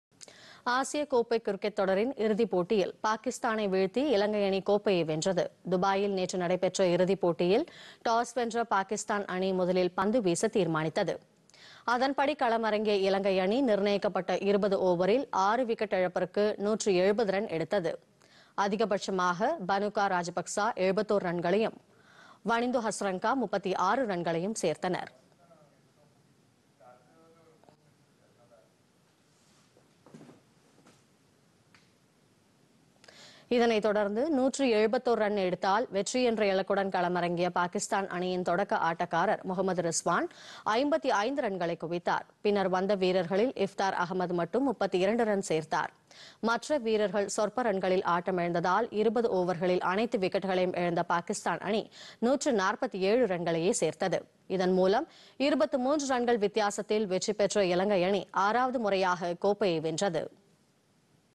ஆசிய கோப்பையை வென்றது இலங்கை
ஆசிய கோப்பை கிரிக்கெட் தொடர் - பாகிஸ்தானை வீழ்த்தி  கோப்பையை வென்றது இலங்கை அணி